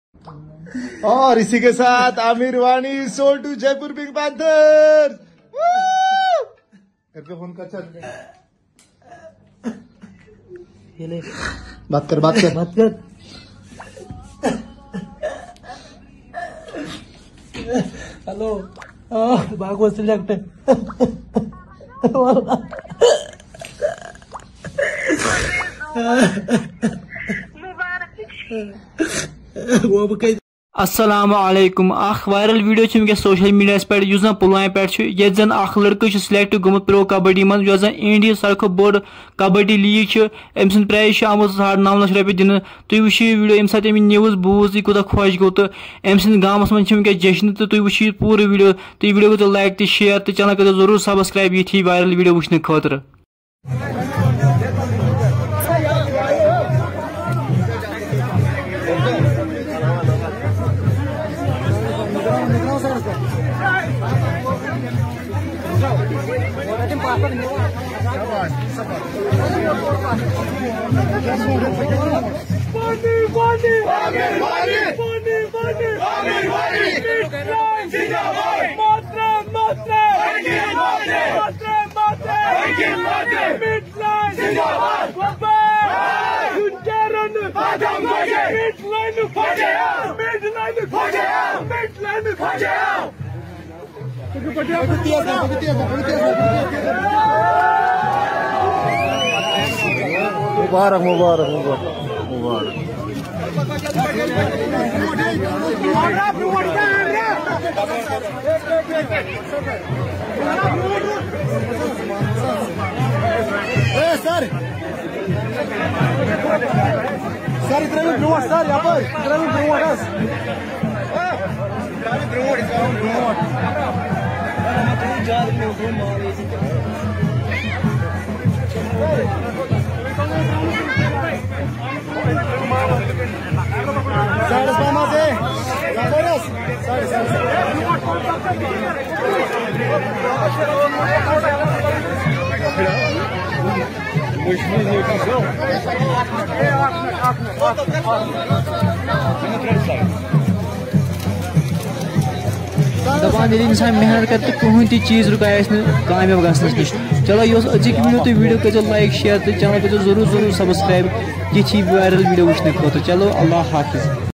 Viral video of Pulwama Boy😭/ Selected For Pro Kabbadi/9 lakh auction Price
amir wani from pulwama selected in pro Kabbadi
amir Wani from pulwama
amir wani selected in Pro Kabbadi
ist time in kashmir